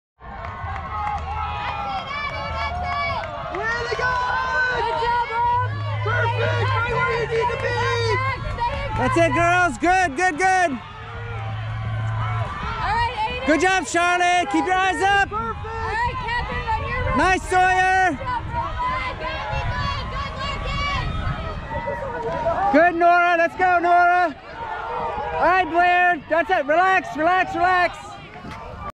20241102 KHSAA State 3A Girls 3
November 2, 2024 KHSAA AAA State Championship Cross County meet at The Kentucky Horse Park in Lexington, KY.